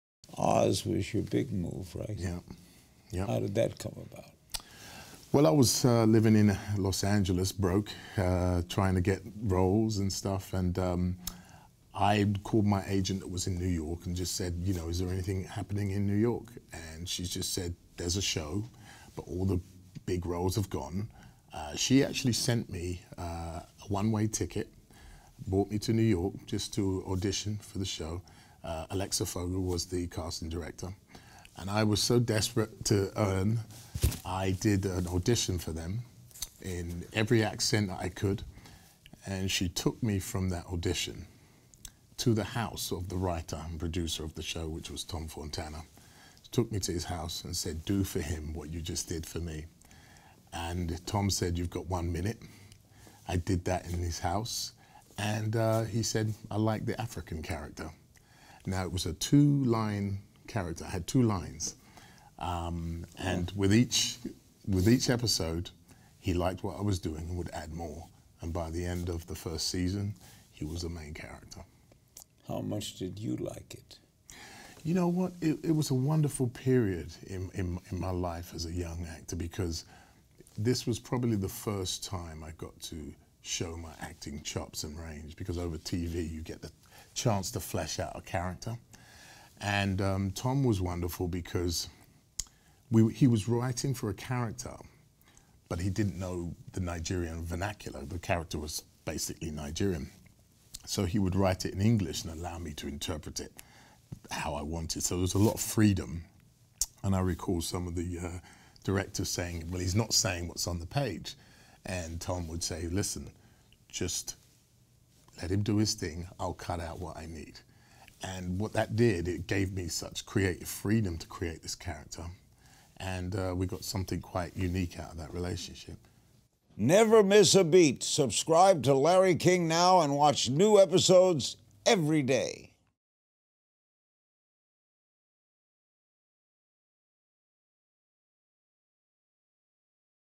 “I was desperate to earn a role”: Adewale Akinnuoye-Agbaje reflects on ‘Oz’
Adewale Akinnuoye-Agbaje remembers his unique audition process for the hit show ‘Oz’ and his desperation to earn a role after coming to Hollywood. Adewale explains how he was able to turn his ‘two line character’ into a series regular.